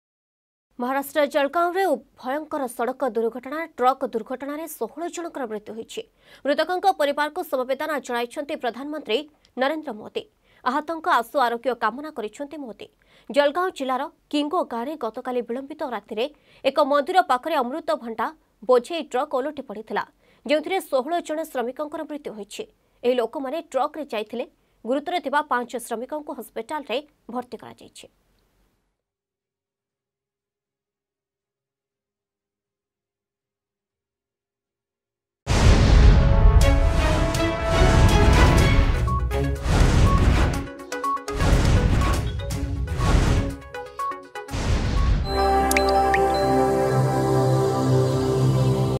ମହାରାଷ୍ଟ୍ର ଜଳଗାଓଁରେ ଭୟଙ୍କର ସଡ଼କ ଦୁର୍ଘଟଣା
ମହାରାଷ୍ଟ୍ର ଜଳଗାଓଁରେ ଭୟଙ୍କର ସଡ଼କ ଦୁର୍ଘଟଣା । ଟ୍ରକ ଦୁର୍ଘଟଣାରେ 16 ଜଣଙ୍କ ମୃତ୍ୟୁ ହୋଇଛି । ମୃତକଙ୍କ ପରିବାରକୁ ସମବେଦନା ଜଣାଇଛନ୍ତି ପ୍ରଧାନମନ୍ତ୍ରୀ ନରେନ୍ଦ୍ର ମୋଦି.
ଅଧିକ ଖବର ପାଇଁ ଡ଼ାଉନଲୋଡ କରନ୍ତୁ “Argus News” ଆପ୍